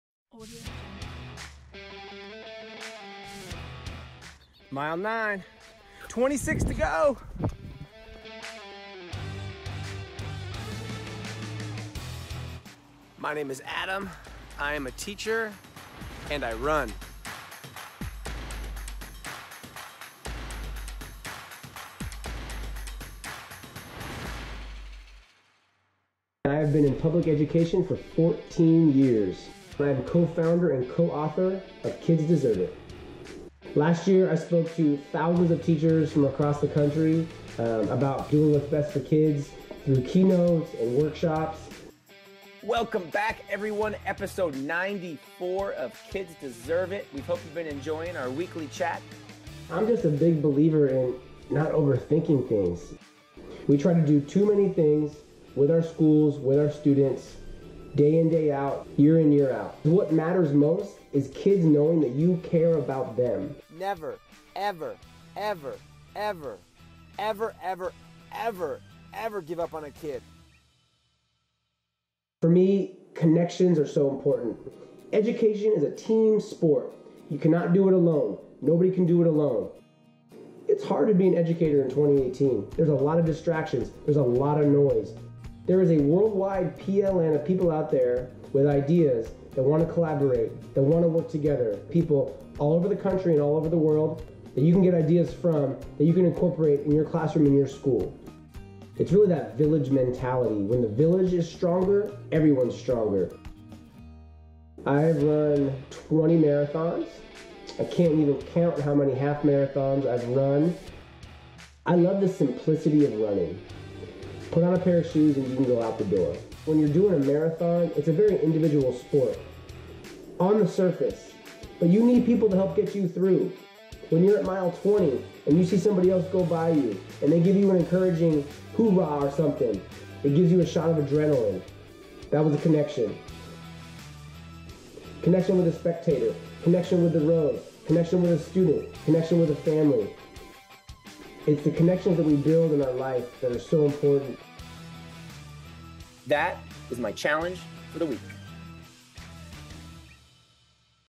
Spotlight on Adam Welcome - Hyland's Boston Marathon 2018 Team
17 Teachers, 1 Boston Marathon Team. These are their stories.

Adam Welcome co-founded Kids Deserve It to spark collaboration between teachers across the country. A seasoned distance runner, the former school principal now relies on the support of others while marathon running. Now preparing for Boston, the lifelong educator from Mt. Diablo Unified School District in Lafayette, CA will lean on the all-teacher HylandsPowered team as he completes his 21st marathon!